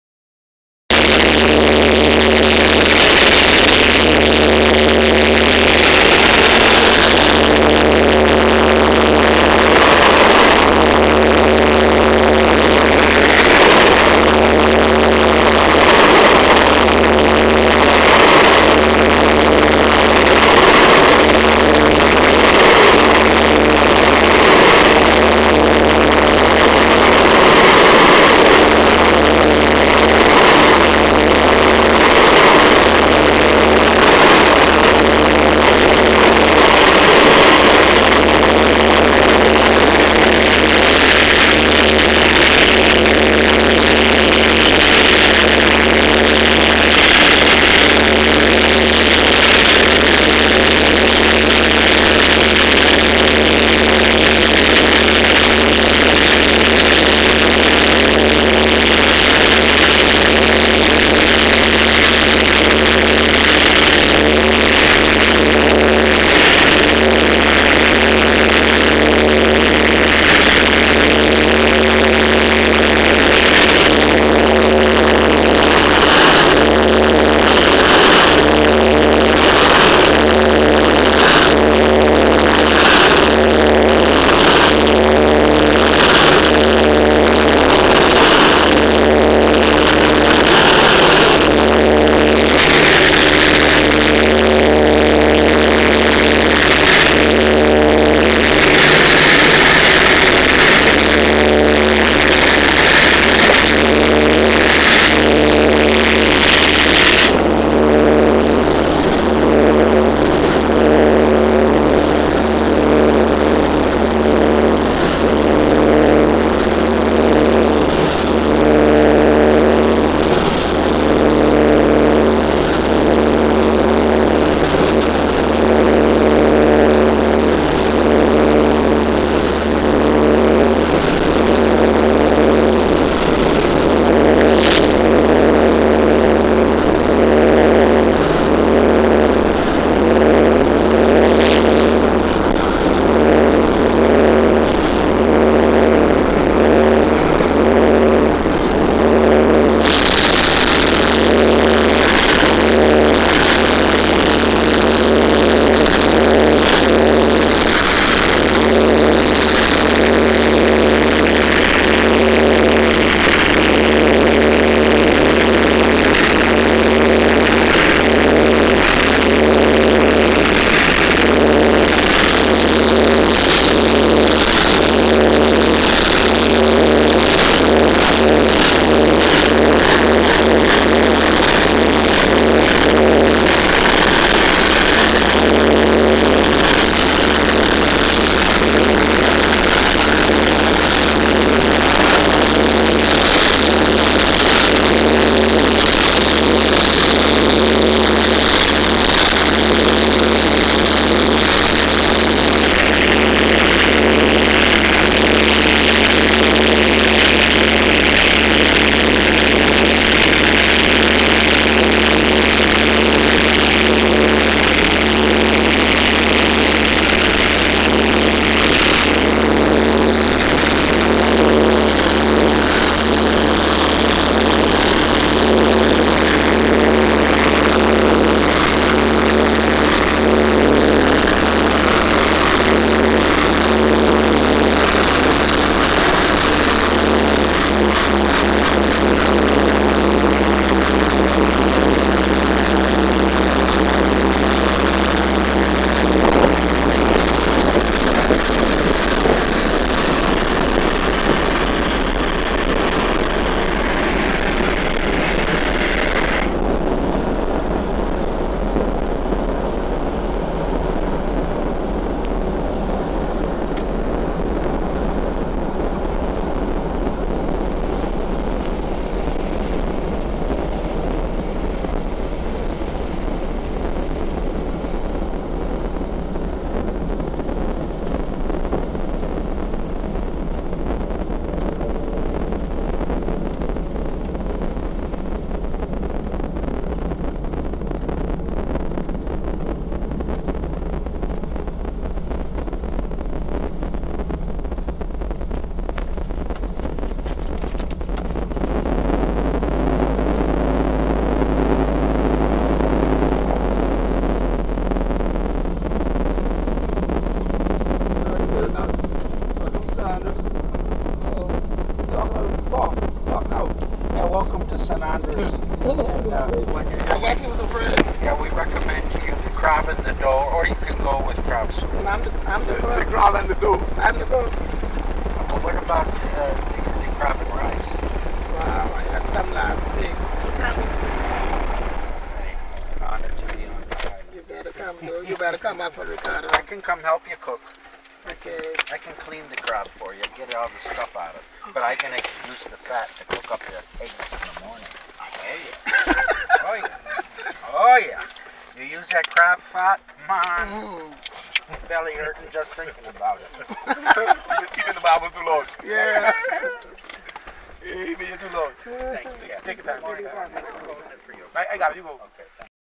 Landing at San Andros Airport
Landing; coming down was some serious turbulance. I was surprised I didn't "shat" myself. I was just too happy when I got out of that plane. It was small and no room at all. I was huddled in the back of a tuna can with wings and my legs were starting to cramp up.